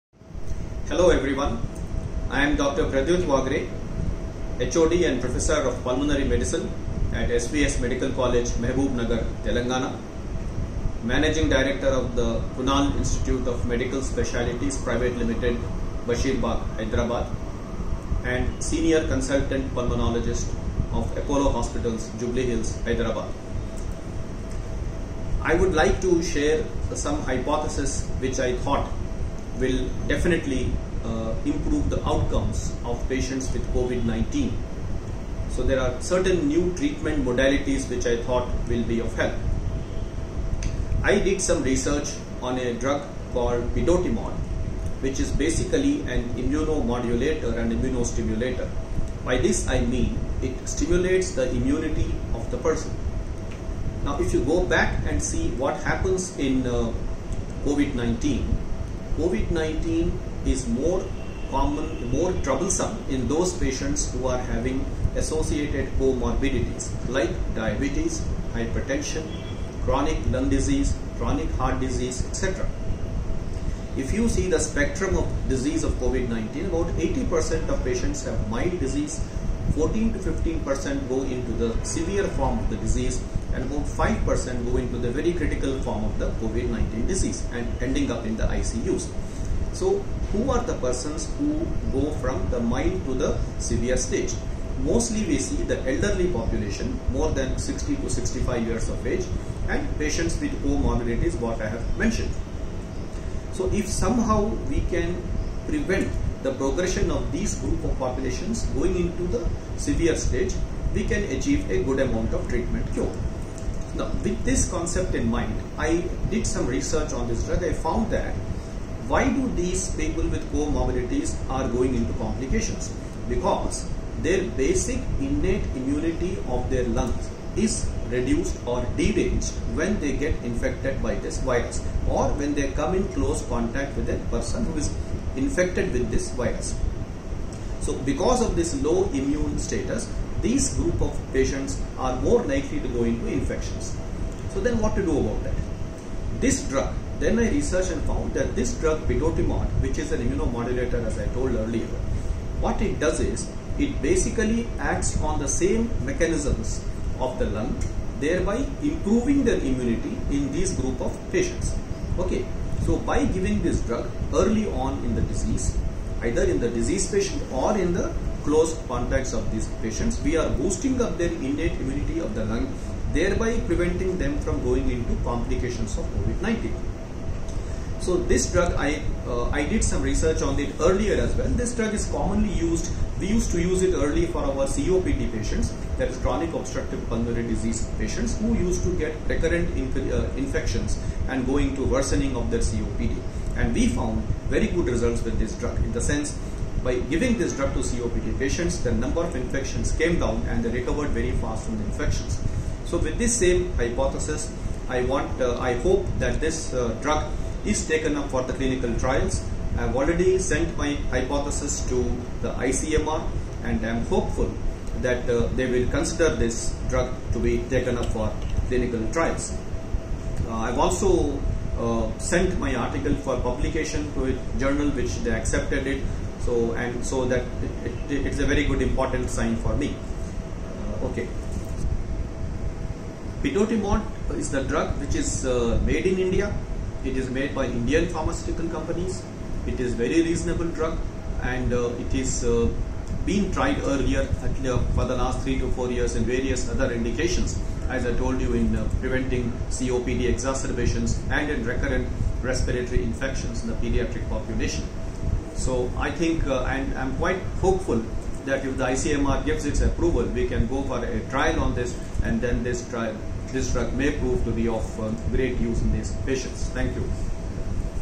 Role of PIDOTIMOD in COVID 19 - Dr.Pradyut Waghray
The video tells about the value of potentiating the inherent immunity in patients of COVID-19 especially those with comorbidities like diabetes, Hypertension, Chronic Lung Diseases, Heart diseases, Chronic Liver diseases, Oncology Patients in preventing the progression of COVID with its attendant complications. PIDOTIMOD is a potent immunomodulator which increases the innate immunity of the individual. In my experience, giving PIDOTIMOD in the early stages of COVID along with the recommended treatment is giving excellent results.
In my next video, I will be speaking on other beneficial drugs in COVID which are commonly available in our country.